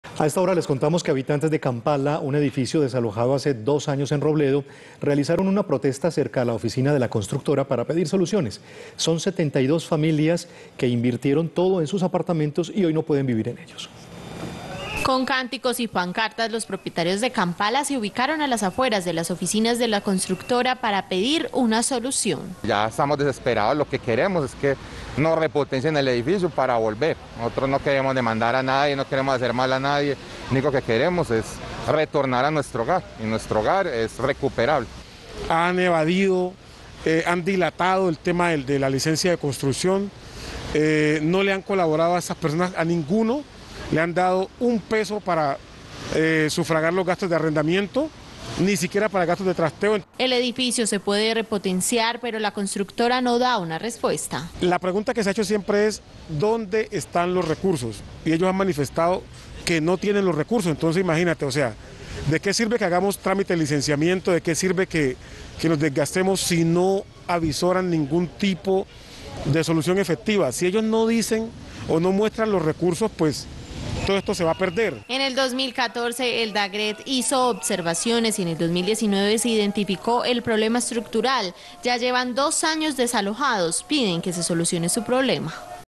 Protestan por repotenciación del Edificio Kampala
Habitantes de Kampala, un edificio desalojado hace dos años en Robledo, realizaron una protesta cerca a la oficina de la constructora para pedir soluciones.

Son 72 familias las que a pesar de haber invertido en sus apartamentos, hoy no pueden vivir en ellos.